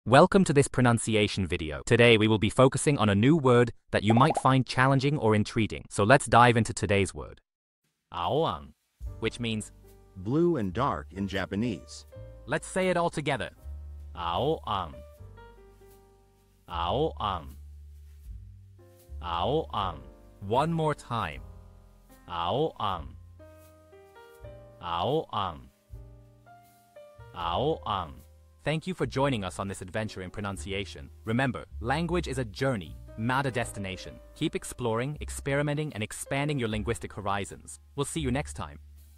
How to pronounce aoan | 青暗 (Blue and dark in Japanese)
Master the Pronunciation of 'aoan | 青暗' - which means : Blue and dark in Japanese 📝 with @PronunciationJapanese 🎓🗣️ - [Your Guide to Japanese]

Learn to pronounce 'aoan' perfectly in Japanese with our easy and engaging tutorial by @PronunciationJapanese! 📖📽️ In this comprehensive video, we provide you with native speaker examples 🗣️👂 and phonetic breakdowns 🎵 to ensure you get the pronunciation just right.